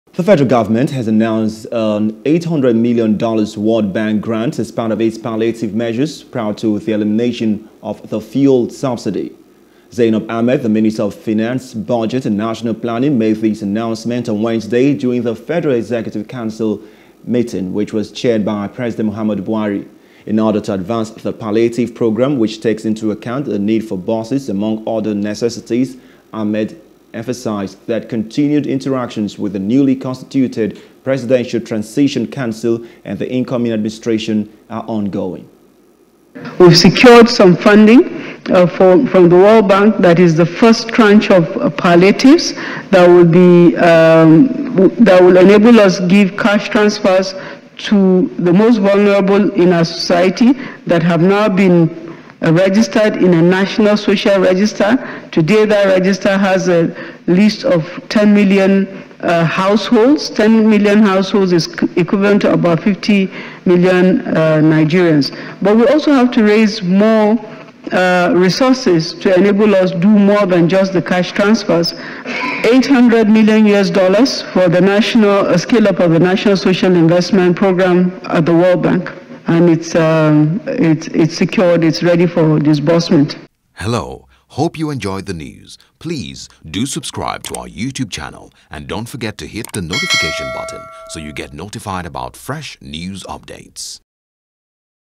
Subsidy Removal: Nigerian Government Gets $800m World Bank Grant For Palliatives
The federal government has announced an $800 million World Bank grant as part of its palliative measures prior to the elimination of the fuel subsidy.